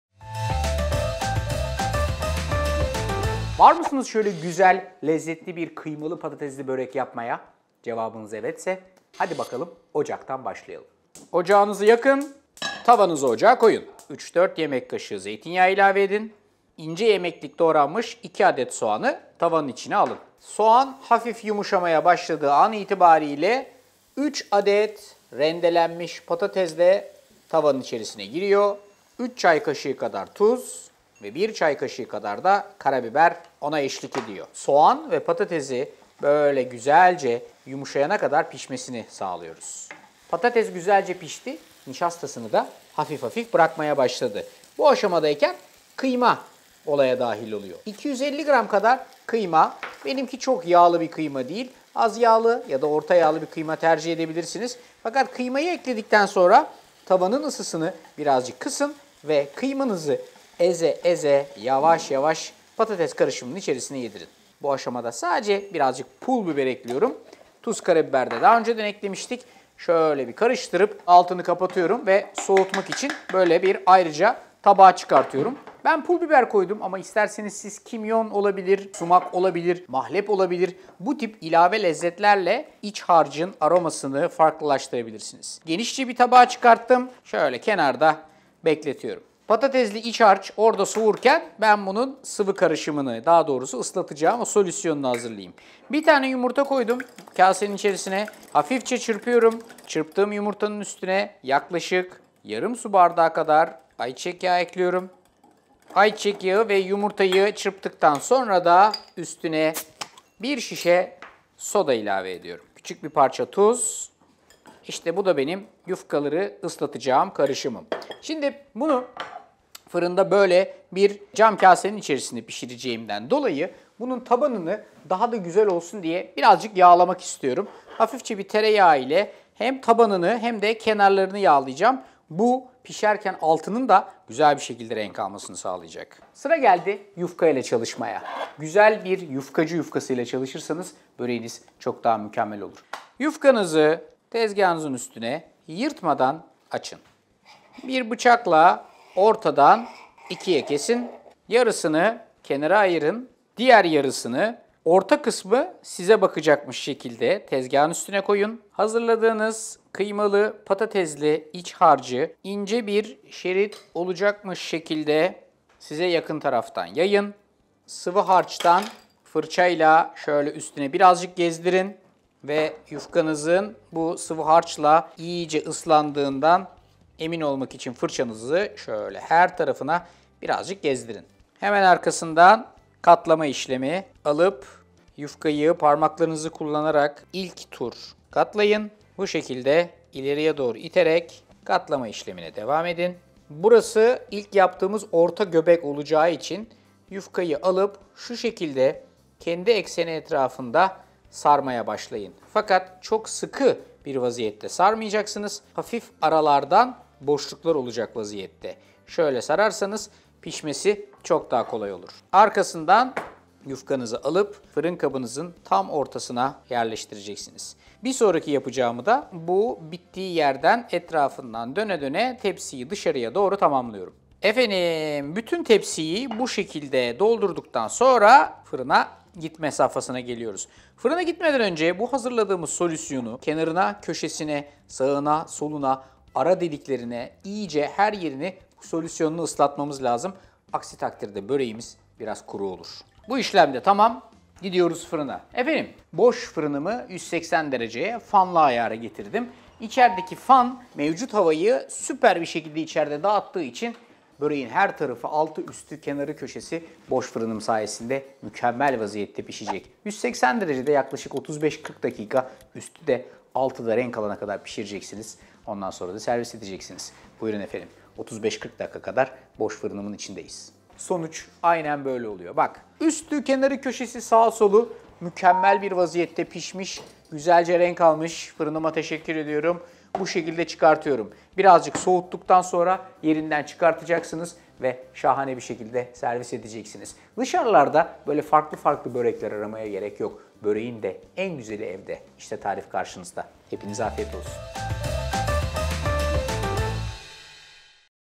Patatesli Kıymalı Börek I Arda'nın Mutfağı
Kıymalı Patatesli Börek
Malzemeler;
2 adet yufka

İç harç için;
3 – 4 yemek kaşığı zeytinyağı
2 adet soğan
3 adet patates – rende
3 çay kaşığı tuz
1 çay kaşığı karabiber
250 gr kıyma
Pul biber

Sıvı harcı için;
1 adet yumurta
½ su bardağı ayçiçek yağı
1 şişe soda – 200 ml
1 tutam tuz

Hazırlanışı;
İç harç için;
Tavanızı ocağa alın ve ısıtın.
Isınan tavanın içerisine zeytinyağı ve ince yemeklik doğradığınız soğanları ilave edip yumuşayana kadar soteleyin.
Sotelenen soğanın üzerine rendelenmiş patatesi ilave edip karıştırın.
Tuz ve karabiber ilave edip kavurun.
Patatesler hafif yumuşayınca kıymayı ilave edin ve kavurmaya devam edin.
Tuz, karabiber ve pul biberi ekleyip patatesler yumuşayıncaya kadar pişirin.
Not: Kıymanız topaklanırsa içerisine 1 yemek kaşığı su ilave edebilirsiniz.

Sıvı harcı için;
Bir karıştırma kabına yumurta, ayçiçek yağı, soda ve tuzu ilave edip bir çırpma teli yardımıyla karıştırın.

Birleştirmek için;
Yufkayı tezgaha alın ve ortadan ikiye kesin.
Yufkanın geniş tarafı kendinize olacak şekilde tezgahınıza yayın.
Hazırladığınız patatesli kıymalı harcı yufkanın uzun olan şeridine 2 parmak kalınlığında yayın.
Yufkanın üst kısmına sıvı harçtan sürerek ıslatın.
Yufkayı iç harçlı kısmından başlayarak rulo olacak şekilde sarın.
Ardından yufkayı tereyağıyla yağladığınız yuvarlak cam fırın kabının ortasına yerleştirin.
Sardığınız yufkaları ortadaki yufkanın etrafına dolayarak yerleştirin.
Böreğinizin üzerine bir fırça yardımıyla kalan sıvı harçtan sürün.
180 derecede önceden ısıtılmış fırında 35 – 40 dakika pişirin.